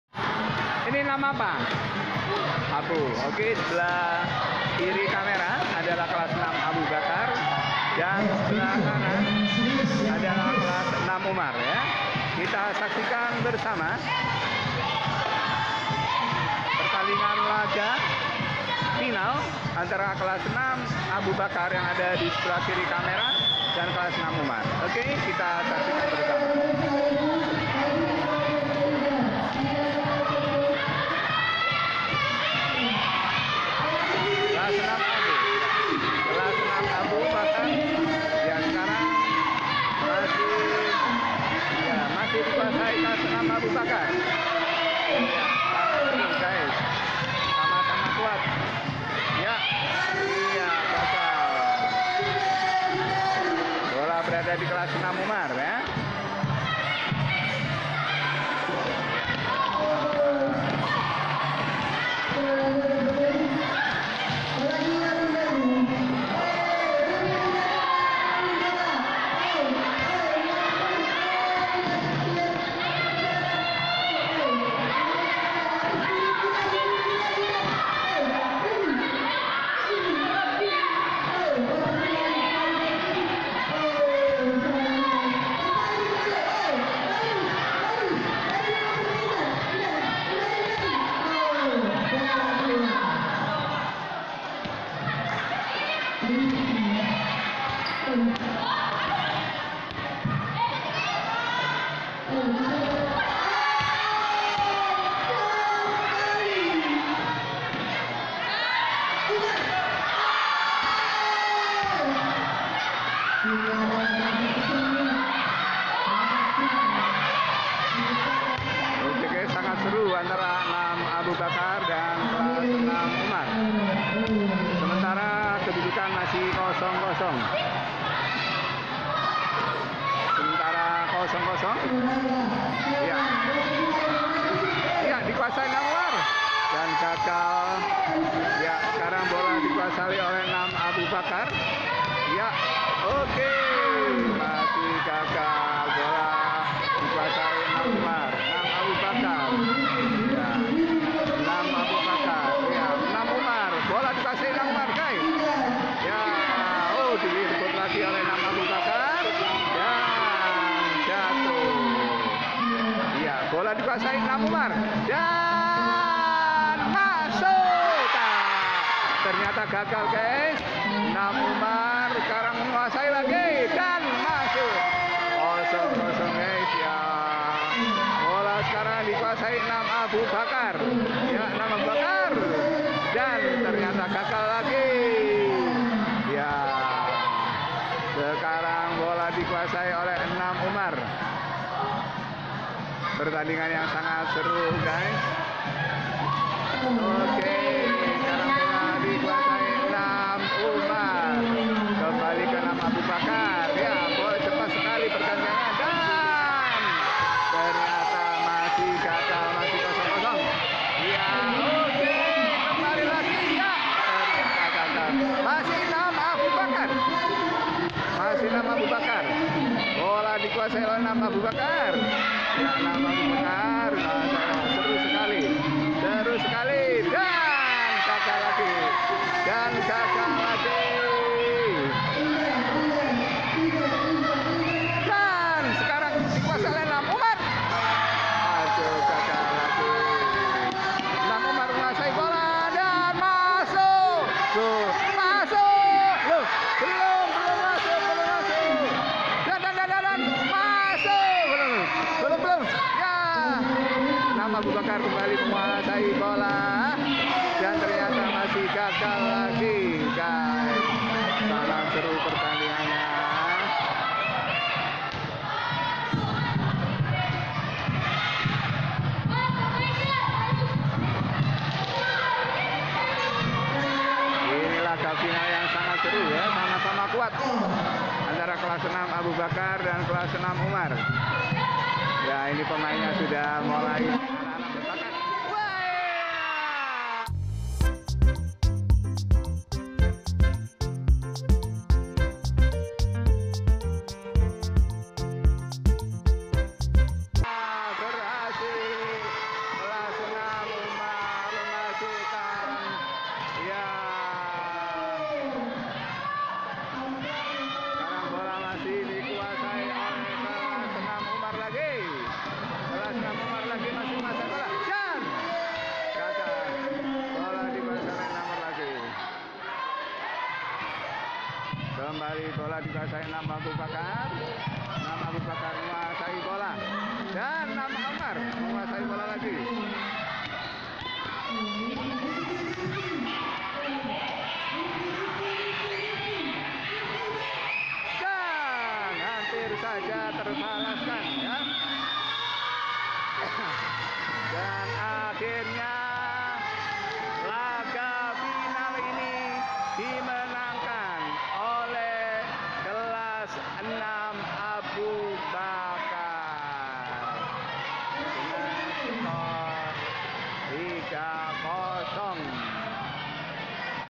Bola basket keren.Skill tingkat dunia.
Dalam rangka Class Meeting di SD Muhida, diadakan laga bola basket.Pertandingan berlangsung sangat seru.Ternyata banyak sekali anak yang berbakat untuk menjadi pemain yang profesional.Selamat menyaksikan.